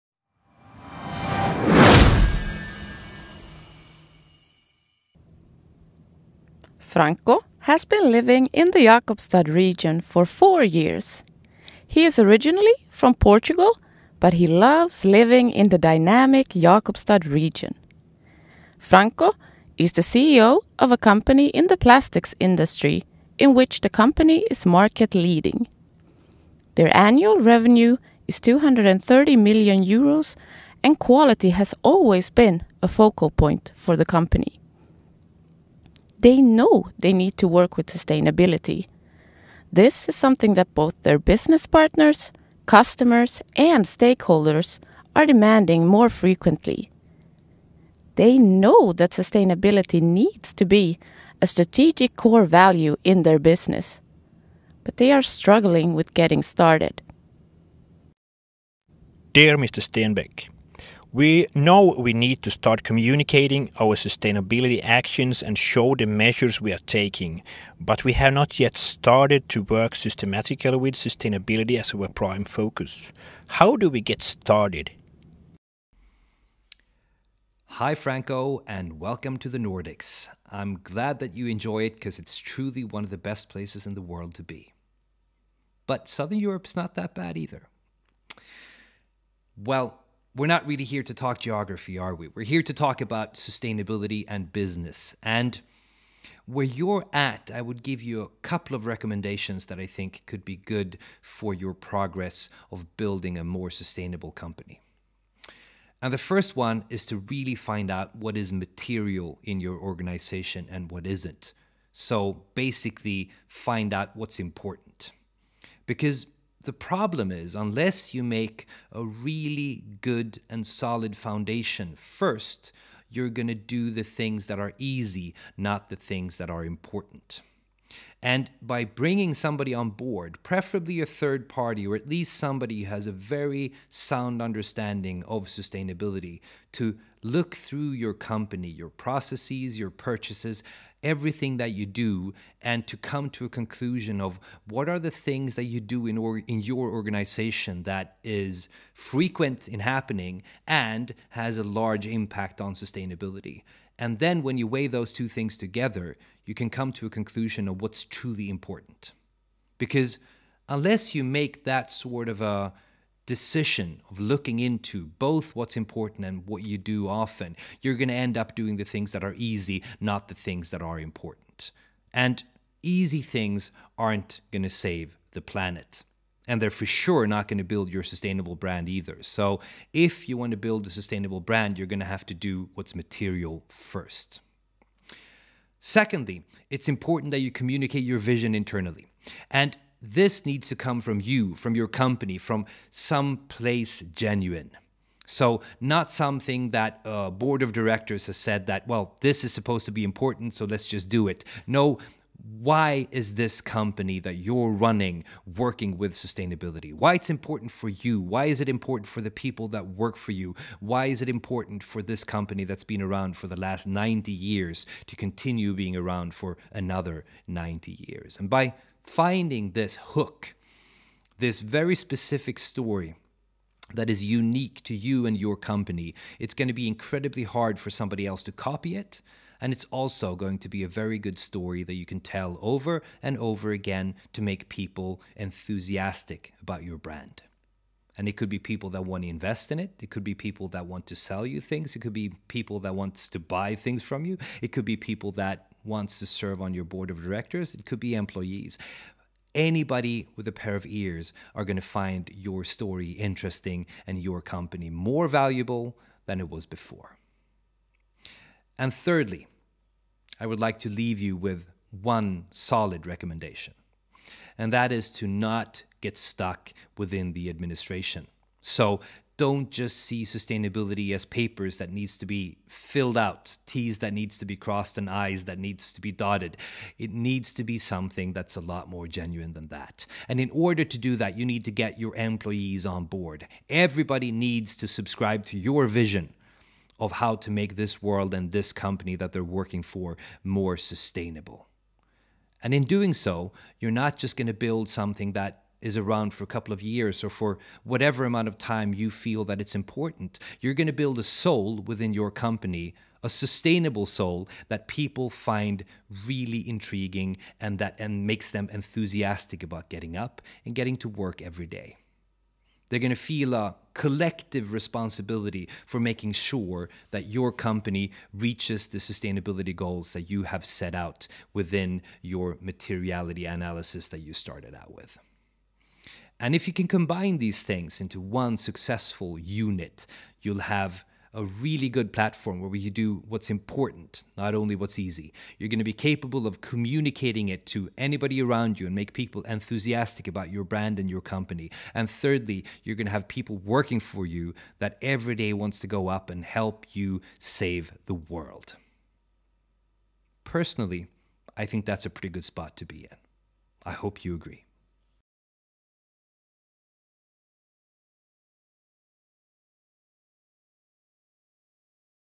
Sustainability challenge: Franco
Say Hi! to Franco a CEO of a company in the plastics industry. Franco is finding it challenging to getting started with their sustainability work. How do we get started and how do we make sustainability our prime focus in the company? Watch this video for what advice the leading Nordic sustainability expert Gustav Stenbeck gives Franco and his team.

This video is the first in a series of three videos (second video will be in Finnish and third in Swedish) where we focus on the most common questions and challenges that companies are facing when working with sustainability in different stages of their sustainability journey.

Pietarsaaren seutu - Maailman paras paikka.
Jakobstadsregionen - Världens bästa plats.
The Jakobstad Region - The best place in the world.